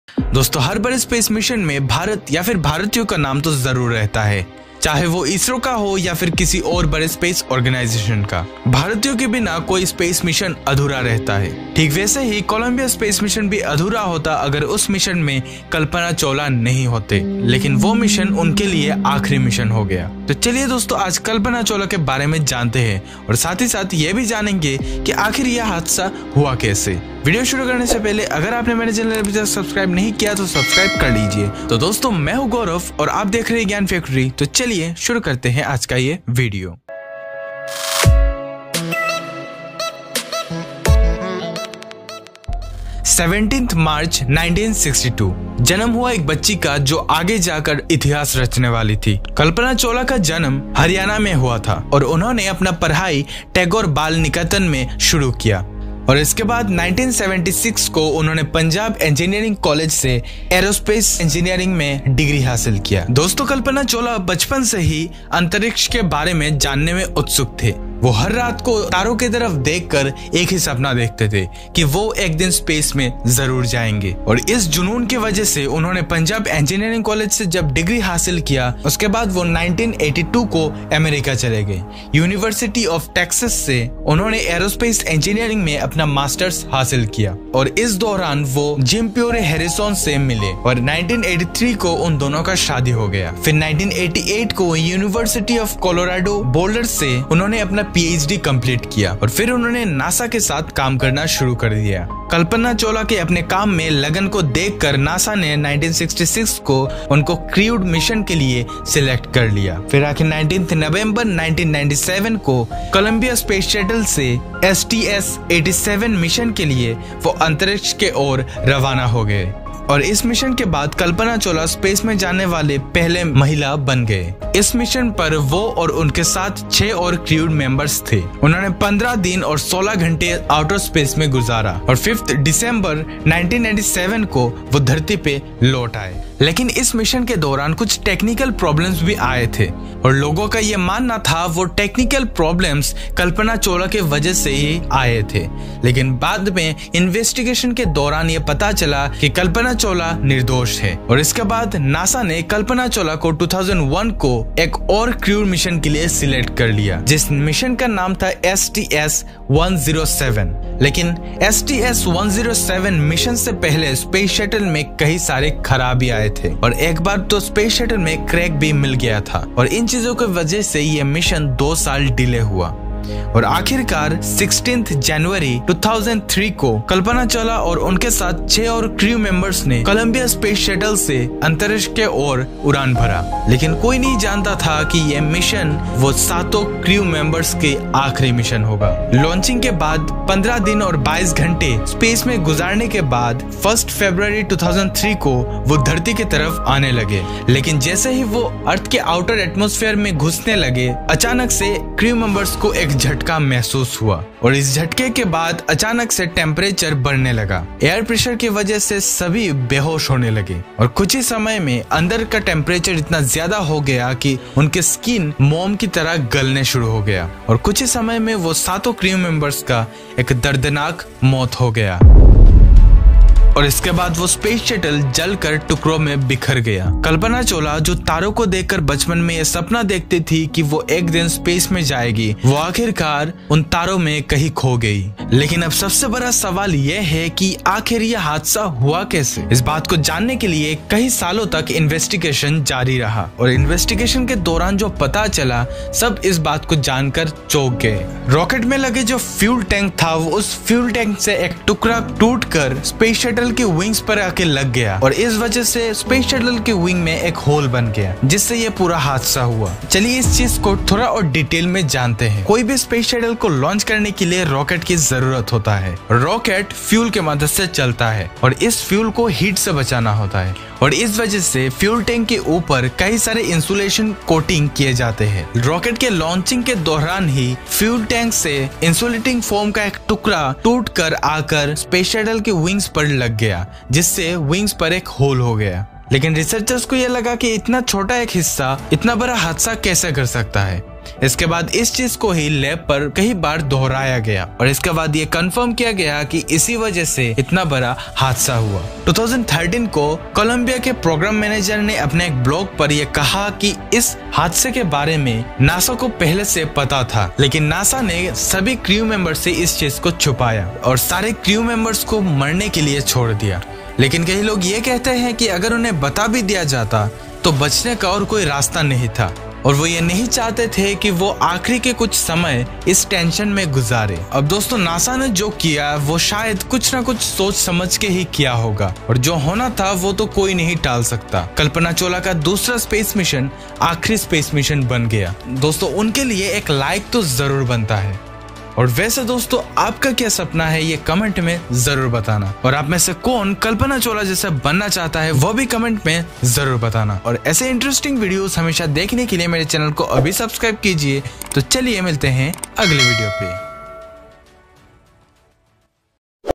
Story of Kalpana Chawla's last mission || How we lost Kalpana Chawla
Welcome to another interesting video

Kalpana Chawla wast the first Indian woman went to space we all know that but how she died ? I have expanded all those things